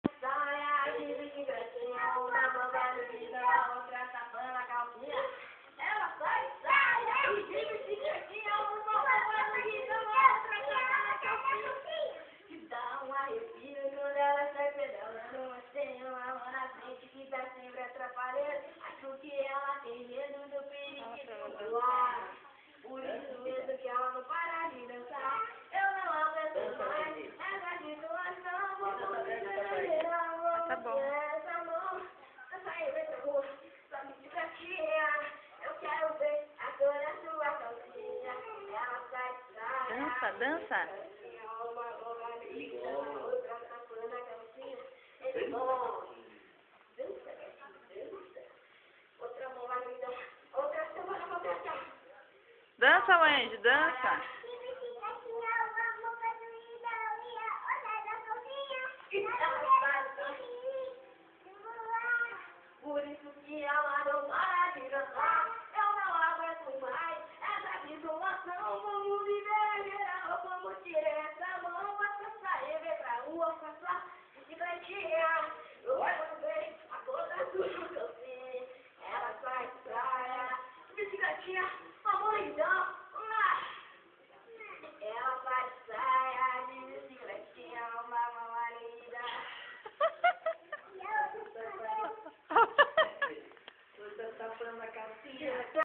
Bicicletinha com os irmãos Willian & Wendy
Este é um flagrante da mamãe Patty nos filhos cantando e dançando juntos.
Willian 11 anos e Wendy 3 anos.